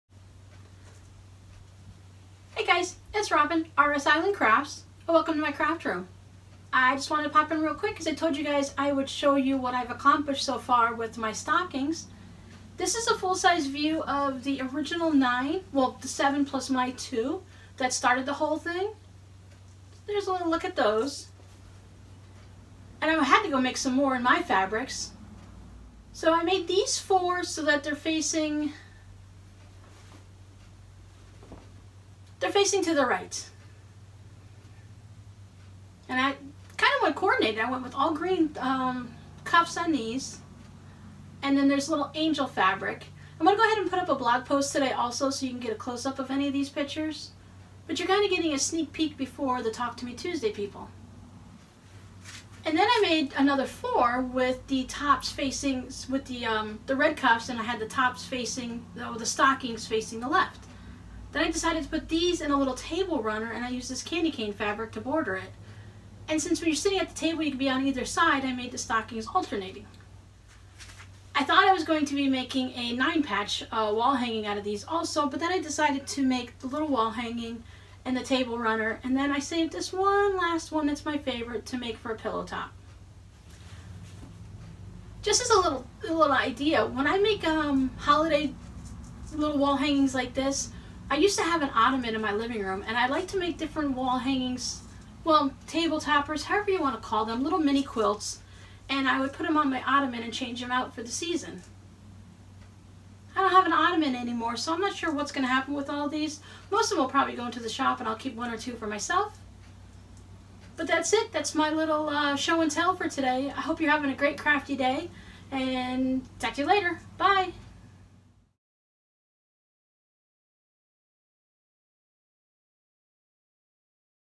Show & Tell Christmas Stocking Block Projects
Make sure you never miss one of my videos. I'll also let you know about new content I'm working on and other interesting updates.

A little sneak peek of some of the projects I am working. You'll see a few of the projects I am working on using the stocking quilt block that I showed this past Friday.

Periscope - as RsIslandCrafts